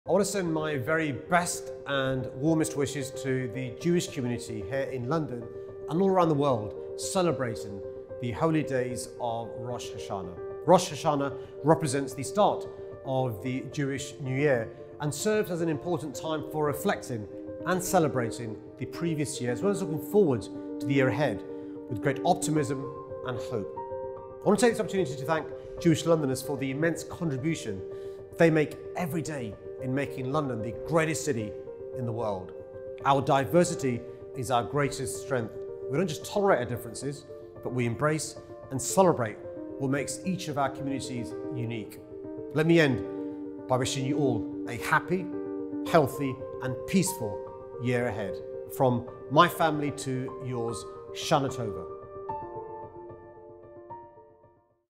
Happy Rosh Hashanah
Rosh Hashanah marks the beginning of the Jewish New Year, a significant celebration for reflecting on the previous year, as well as preparing to look forward to the year ahead with joy and anticipation.

I’d also like to highlight and give thanks to the importance of the Jewish London community and their contribution in strengthening the key values of our city—our diversity only makes us stronger. Wishing you a joyous and successful year, Shanah Tovah.